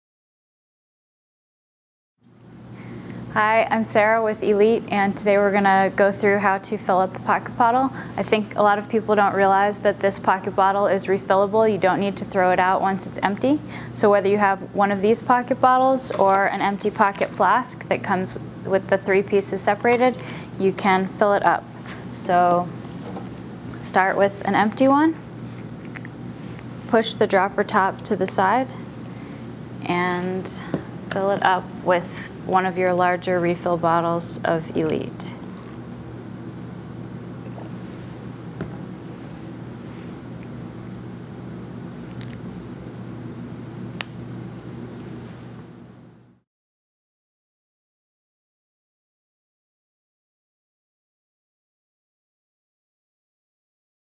How to Refill the elete Electrolytes Pocket Bottle
Mountain bike endurance champion Sarah Kaufmann demonstrates how easy it is to use and refill the convenient elete 24.6-ml pocket bottle.